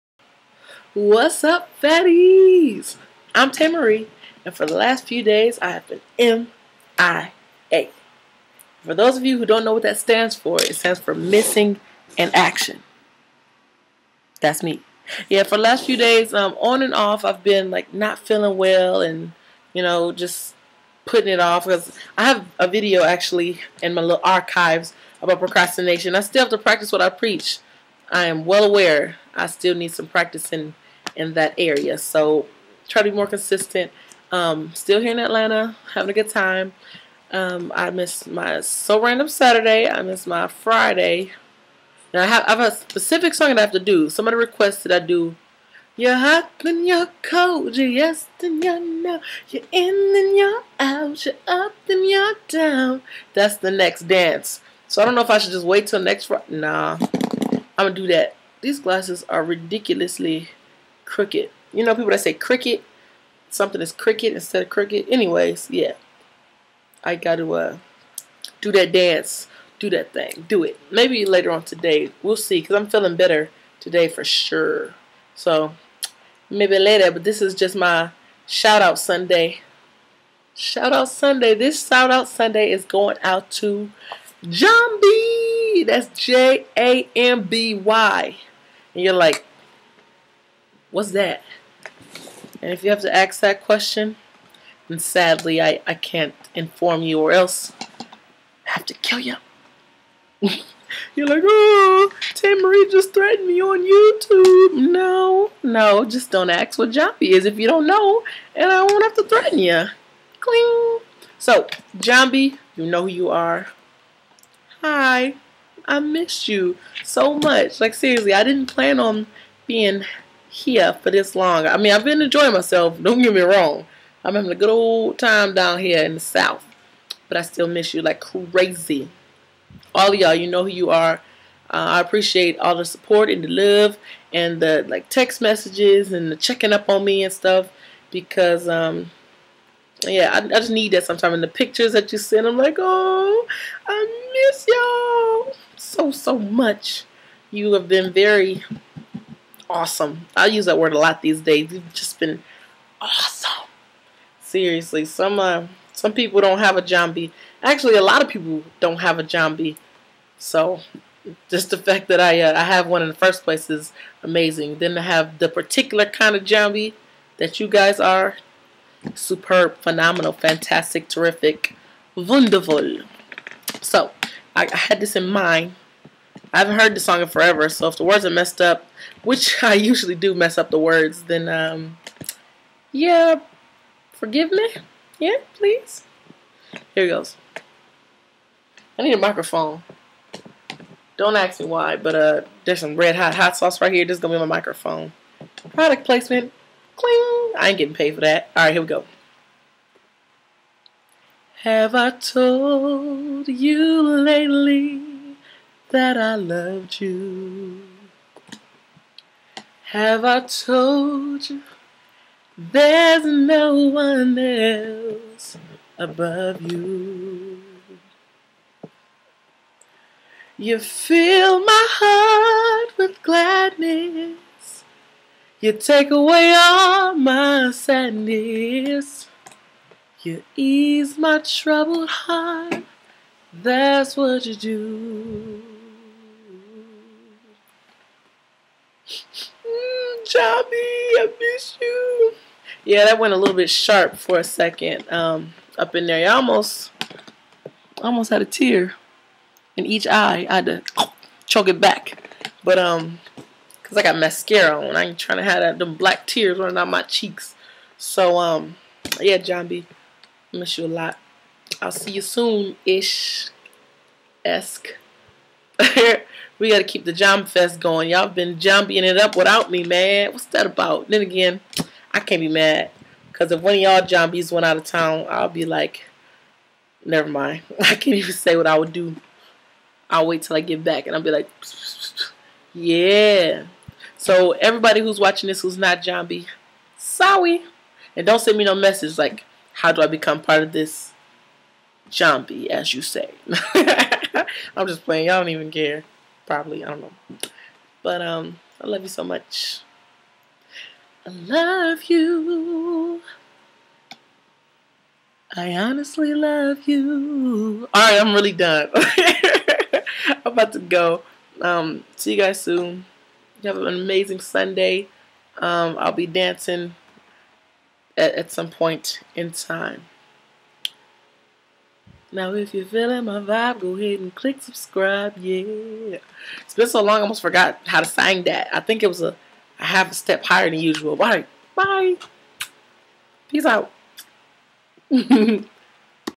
SHOUT-OUT SUNDAY: JAMBY EDITION!*Don't know? Don't ask!* :-P
This is a shout-out to the elite members of my JAMBY! Love ya so much... enjoy my singin/mini-dancing random vid! :)
Much love,
Tae-marie